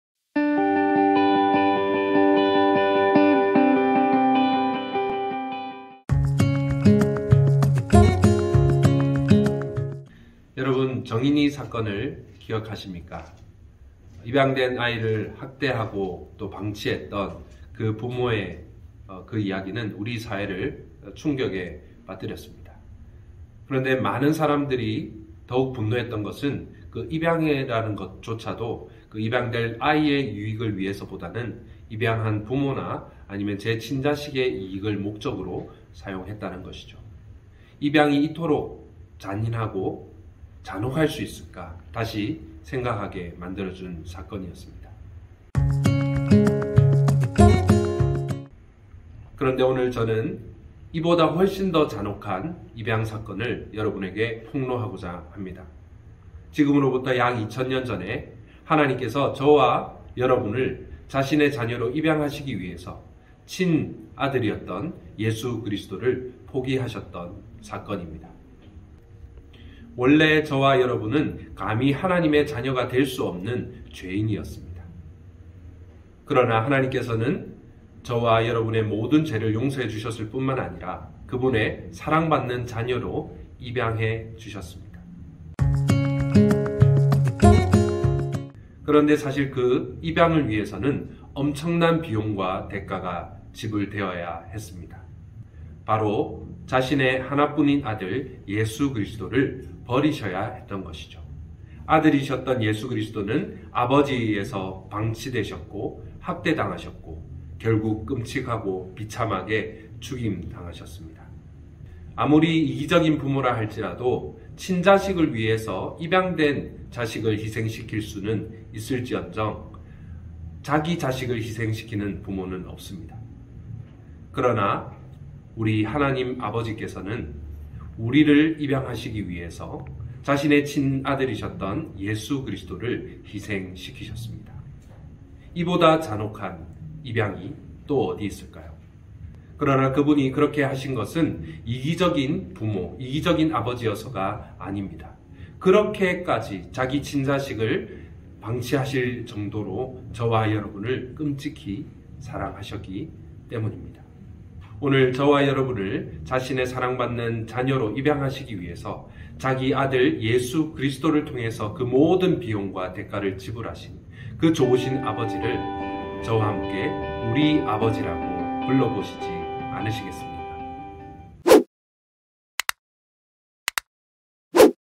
가스펠톡 Gospeltalk Ep.04 l 잔혹한 입양 Cruel Adoption (English subtitles)
기독교의 복음이 무엇이며, 그것이 어떻게 우리의 삶과 도시를 변화시키는지에 대해서 알아보는 가스펠톡 시리즈 네 번째 영상입니다.
This is the fourth video of Gospeltalk series that talk about what the gospel of Christianity is and how it transforms our lives and the city.

오늘 다룰 주제는 다음과 같습니다:
Today's theme is as followings:

1. 오늘 우리가 하나님의 사랑받는 자녀가 된 것은 하나님께서 우리를 입양하셨기 때문이다.
God has adopted us so that we can become His beloved children.

2. 하나님께서는 우리를 입양하시기 위해 자신의 외아들 예수 그리스도를 통해 큰 대가를 지불하셨다.
God paid the huge price through His One and Only Son Jesus Christ to adopt us as His beloved children.